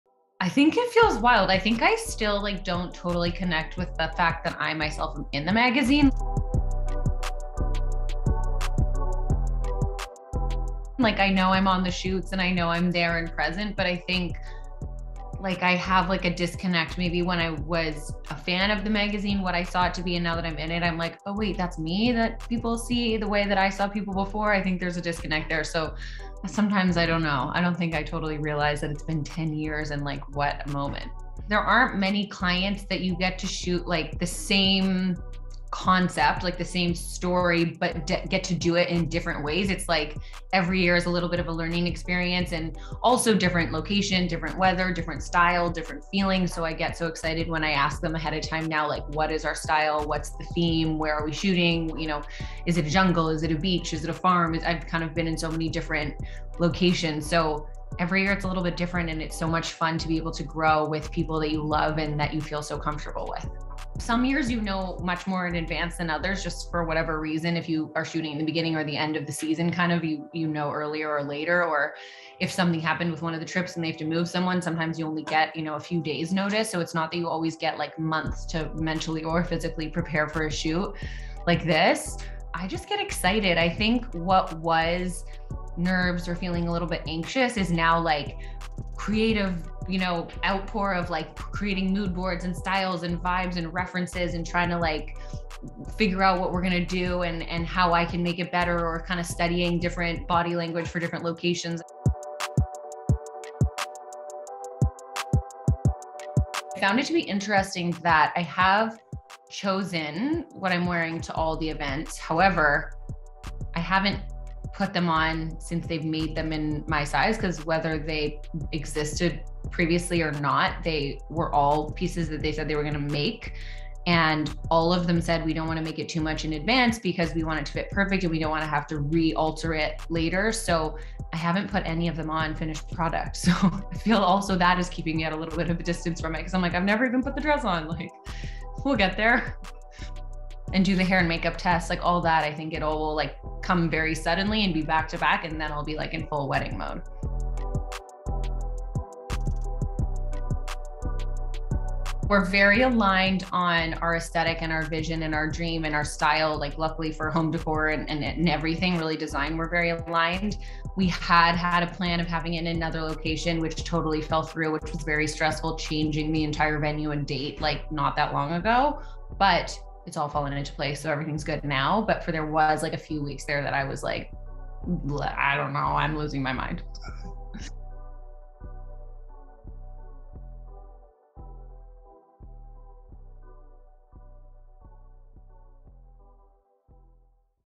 Kate Bock on 10 years of posing for the Sports Illustrated Swimsuit issue | Page Six Celebrity News
Kate Bock is a Sports Illustrated Swimsuit issue staple — but she’s not slowing down any time soon.

The model makes her 10th appearance in SI Swim for 2022, marking a decade-long run sporting teeny bikinis for the mag. But that doesn’t mean it’s gotten any easier.

“It feels wild. I still don’t totally connect with the fact that I myself am in the magazine,” Bock tells Page Six Style.

But even a 10-year pro can continue learning, and Bock, 34, tells us she’s now become so close with the SI Swimsuit team that she works collaboratively on the concept, mood board and creative styling for her shoots as much as possible.